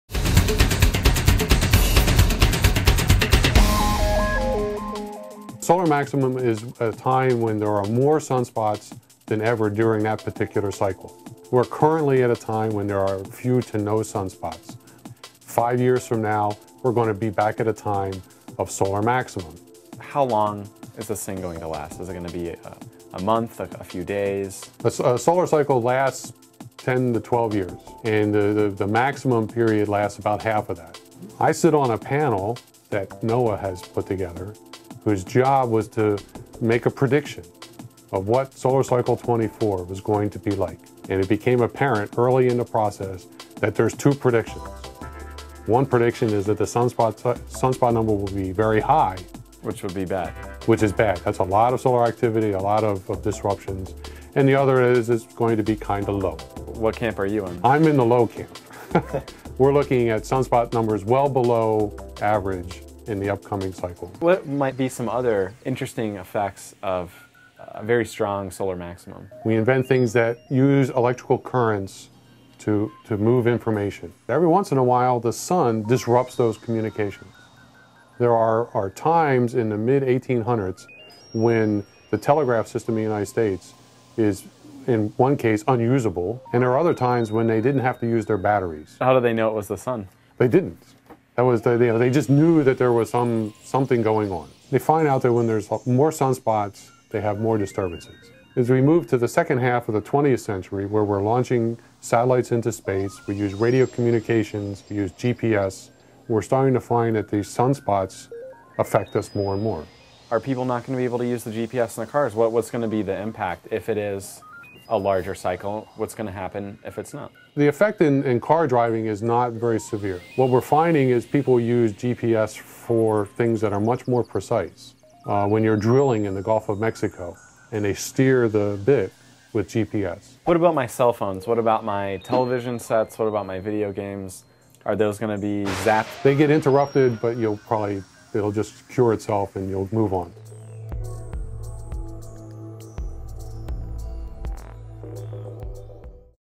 Solar Maximum
Discovery-News.com; What is the solar maximum and how will it affect everything from orbiting satellites to mobile phones? Discovery Channel's Dave Mosher finds out. Jorge Ribas produces.